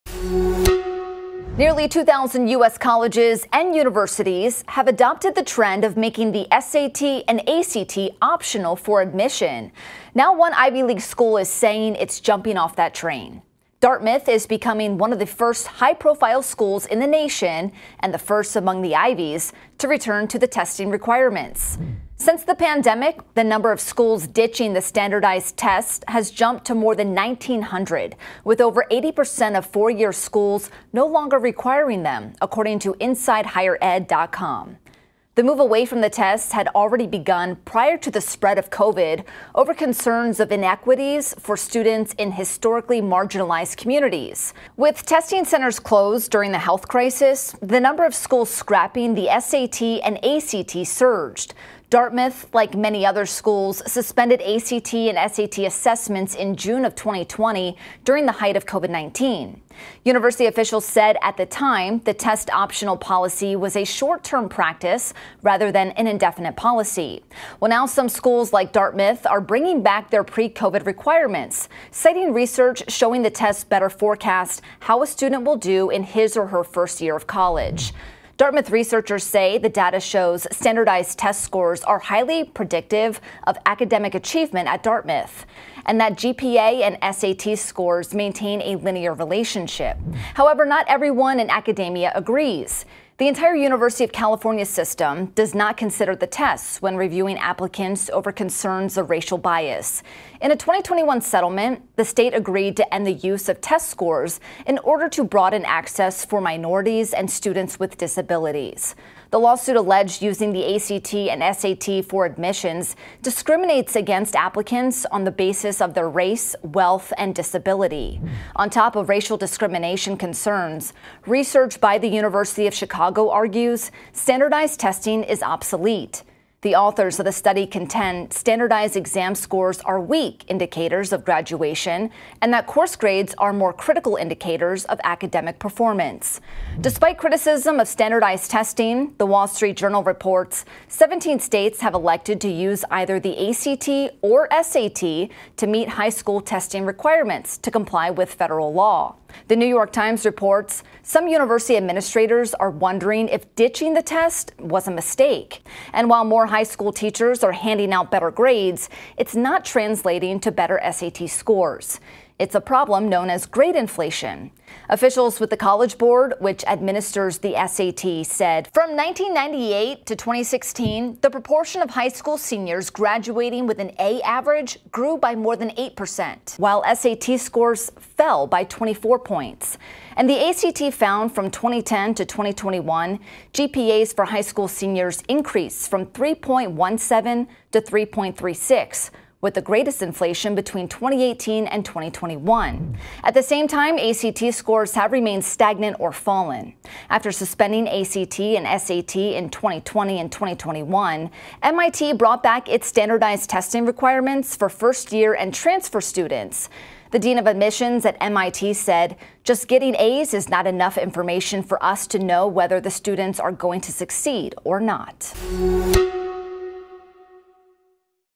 As nearly 2,000 universities scrap ACT and SAT, Dartmouth reverses course
Nearly 2,000 U.S. colleges and universities have made the SAT and ACT optional for admission. However, Dartmouth University recently announced that it is reinstating the requirement of submitting ACT and SAT scores for admission. The renewed requirement will begin for the class of 2029.